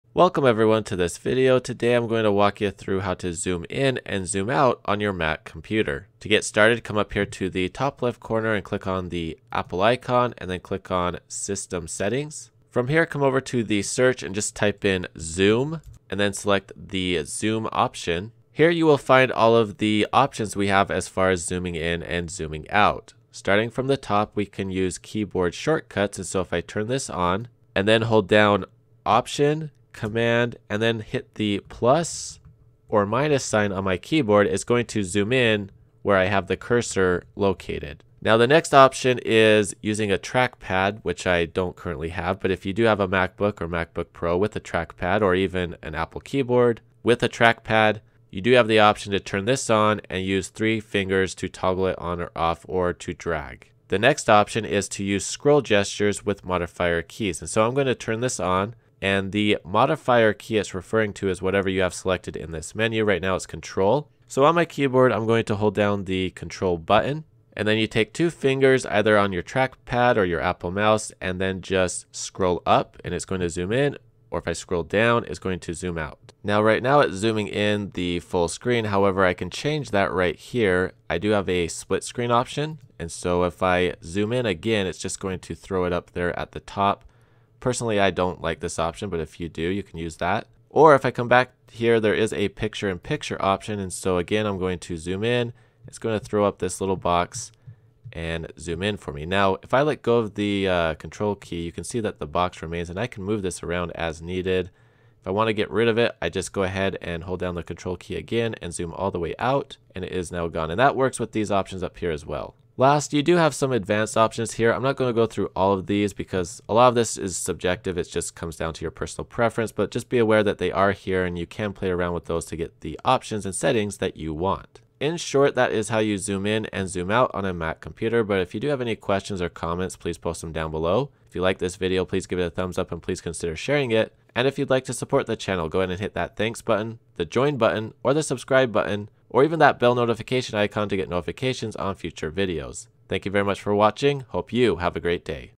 All The Ways You Can Zoom In & Zoom Out In macOS | Zoom Settings | A Quick & Easy Guide
Learn how to zoom in and zoom out on a Mac. This shows a user where all the zoom settings are located, how to zoom in and out, and how to adjust between full screen, split screen, and picture in picture.

M2 Mac 2023
MacBook Pro

M2 Mac 2022
Macbook Pro